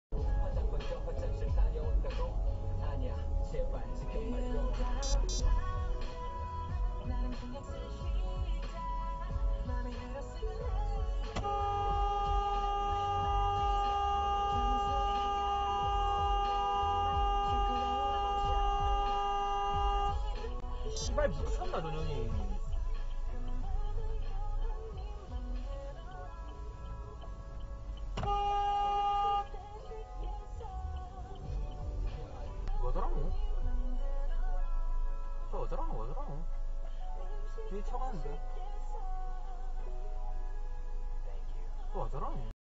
실연당한여자인가..도로위망연자실 / 블랙박스TV
도로위에서 모든걸잃은표정으로걷고있죠,무슨사연인진몰라도위험천만하넹ᆢㄷ